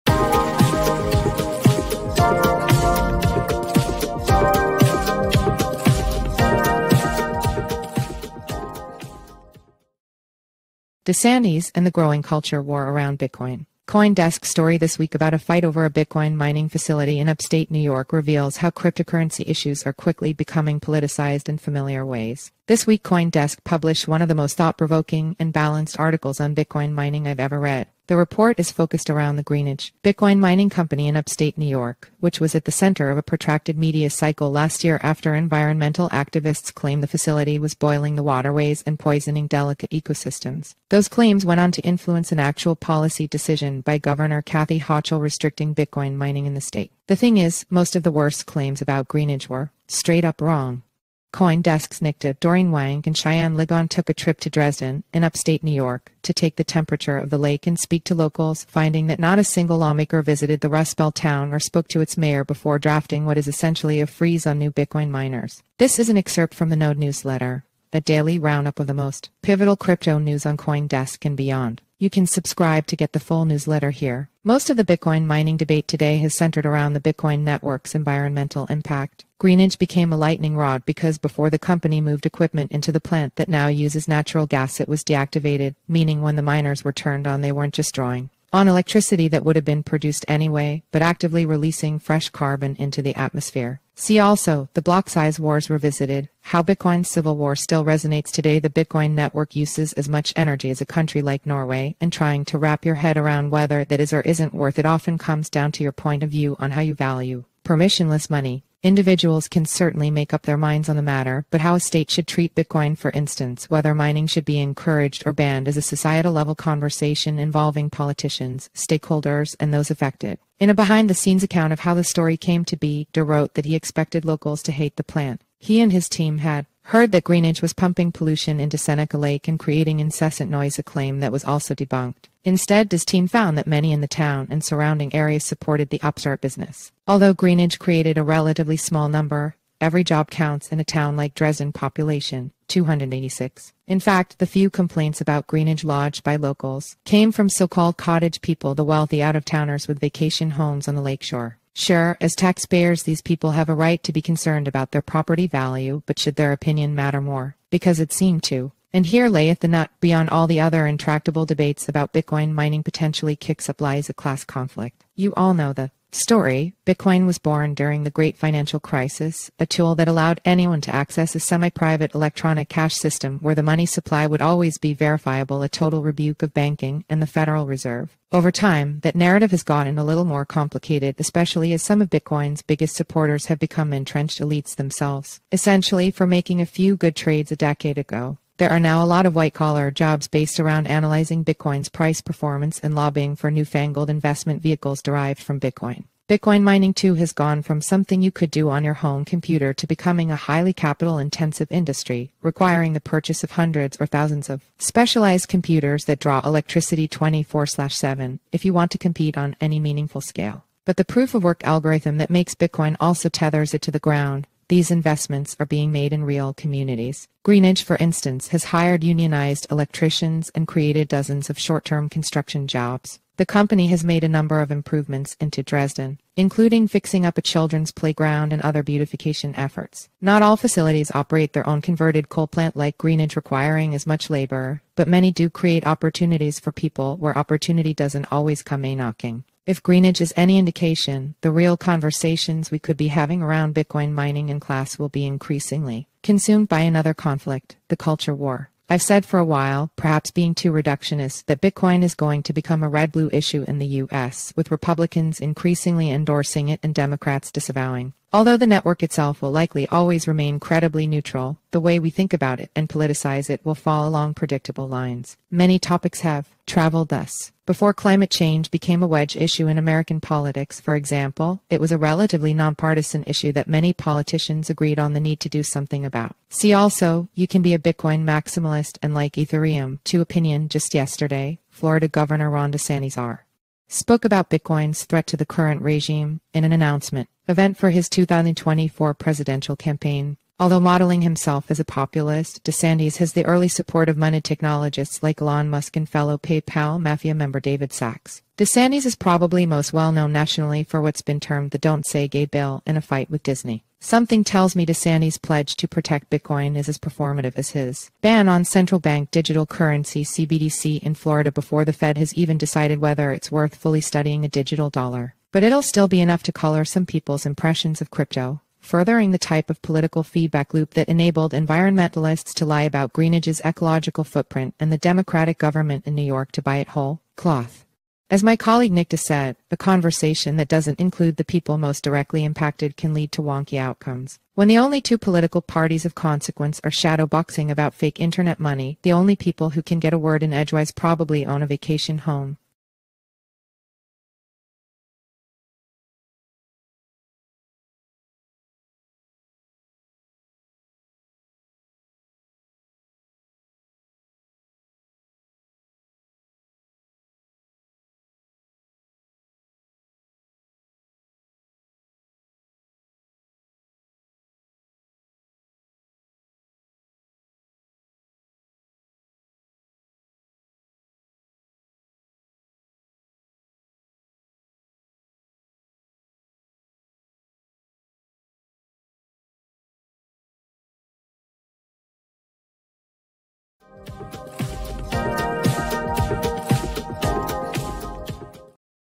DeSantis and the Growing Culture War Around Bitcoin
CoinDesk’s story this week about a fight over a bitcoin mining facility in Upstate New York reveals how cryptocurrency issues are quickly becoming politicized in familiar ways.

This week CoinDesk published one of the most thought-provoking and balanced articles on bitcoin mining I’ve ever read. The report is focused around the Greenidge bitcoin mining company in upstate New York, which was at the center of a protracted media cycle last year after environmental activists claimed the facility was boiling the waterways and poisoning delicate ecosystems. Those claims went on to influence an actual policy decision by Governor Kathy Hochul restricting bitcoin mining in the state.

The thing is, most of the worst claims about Greenidge were straight up wrong. CoinDesk’s Nik De, Doreen Wang and Cheyenne Ligon took a trip to Dresden, in Upstate New York, to take the temperature of the lake and speak to locals, finding that not a single lawmaker visited the rust-belt town or spoke to its mayor before drafting what is essentially a freeze on new bitcoin miners.

This is an excerpt from The Node newsletter, a daily roundup of the most pivotal crypto news on CoinDesk and beyond.

Most of the bitcoin mining debate today has centered around the Bitcoin network’s environmental impact. Greenidge became a lightning rod because before the company moved equipment into the plant that now uses natural gas it was deactivated, meaning when the miners were turned on they weren’t just drawing on electricity that would have been produced anyway, but actively releasing “fresh” carbon into the atmosphere.

See also: The Blocksize Wars Revisited: How Bitcoin's Civil War Still Resonates Today

The Bitcoin network uses as much energy as a country like Norway, and trying to wrap your head around whether that is or isn’t “worth it” often comes down to your point of view on how you value permissionless money. Individuals can certainly make up their minds on the matter, but how a state should treat Bitcoin – for instance, whether mining should be encouraged or banned – is a societal-level conversation involving politicians, stakeholders and those affected.

In a behind the scenes account of how the story came to be, De wrote that he expected locals to hate the plant. He and his team had heard that Greenidge was pumping pollution into Seneca Lake and creating incessant noise (a claim that was also debunked). Instead, De’s team found that many in the town and surrounding area supported the upstart business. Although Greenidge created a relatively small number, every job counts in a town like Dresden (population: 296).

In fact, the few complaints about Greenidge lodged by locals came from so-called “cottage people” – the wealthy out-of-towners with vacation homes on the lakeshore. Sure, as taxpayers these people have a right to be concerned about their property value, but should their opinion matter more? Because it seemed to.

And here layeth the nut: Beyond all the other intractable debates about bitcoin mining potentially kicks up lies a class conflict. You all know the story: Bitcoin was born during the Great Financial Crisis, a tool that allowed anyone to access a semi-private electronic cash system where the money supply would always be verifiable – a total rebuke of banking and the Federal Reserve.

Over time, that narrative has gotten a little more...